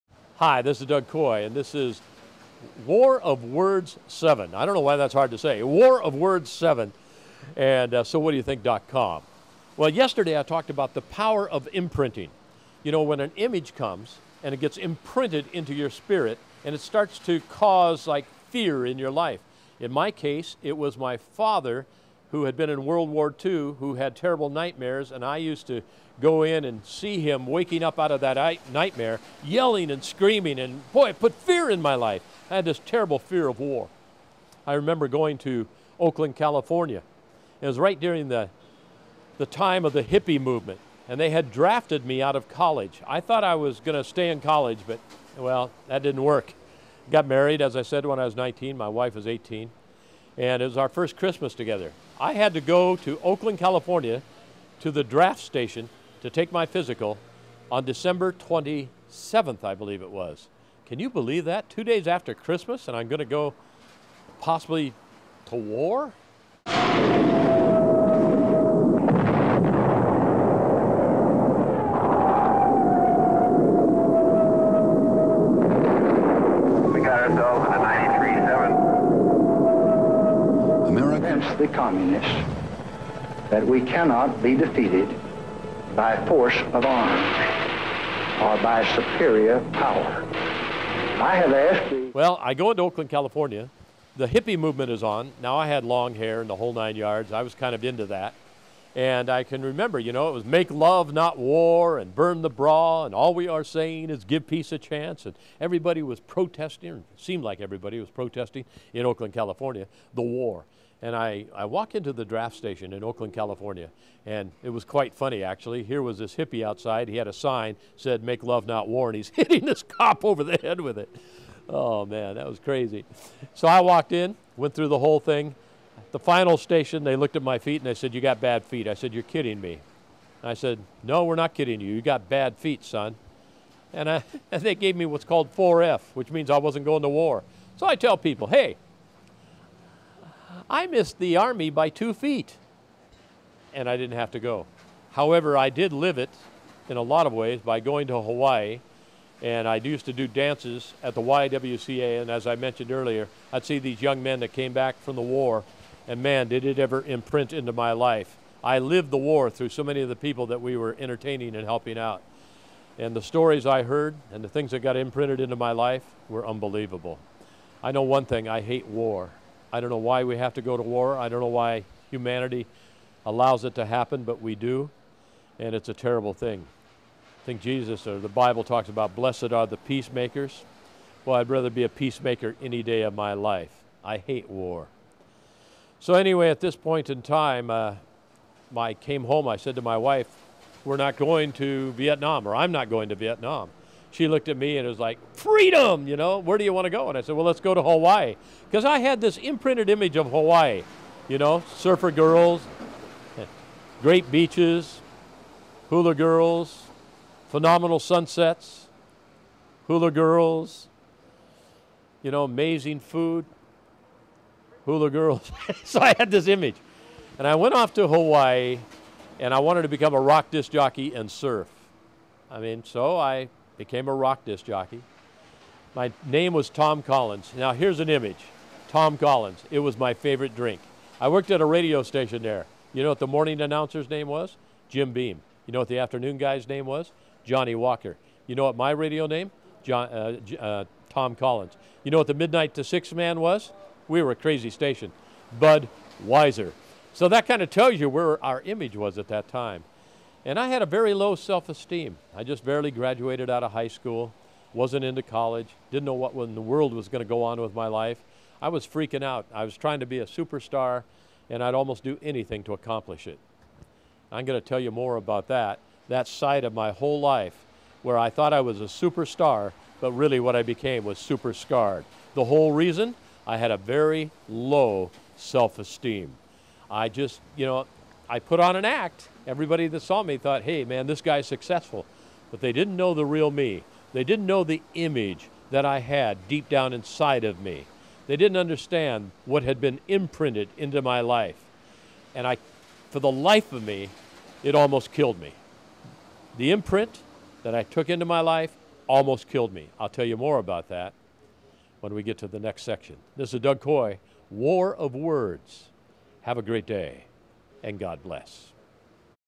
The War Of Words 7
War is hell!  This is a true statement.  The tragic loss of young people who fight for their countries and make the ultimate sacrifice is so sad.  No wonder the Bible says, "blessed are the peacemakers."
I have been talking about the war of words.  It is words that create wars and words that bring peace.  A speech can encourage a nation or it can incite hatred.  When Martin Luther King Jr. said, "I have a dream." it inspired a group of people to rise up out of slavery and bigotry and they lived to see the dream he spoke of become a reality.

On the other hand you can have Hitler speak his hatred for the Jews and then follow-up his hatred with unbelievable actions that resulted in Jews being killed on a daily basis by the thousands.  Who was the one who inspired Hitler?  It was the theologian Martin Luther who wrote the pamphlet, "The Christ Killers."   This pamphlet and the words of Martin Luther had a profound affect on Hitler and his world view.

Words on a personal level can challenge us to great heights of achievement, or they can bring us to the darkest moments of our lives.  The question is what words are you listening to.  Words are containers and they carry faith or fear.  So faith comes by hearing, and so does fear.  The secret to life is who are you listening to and what words are forming you.  It really is a matter of life or death.  So what do you think?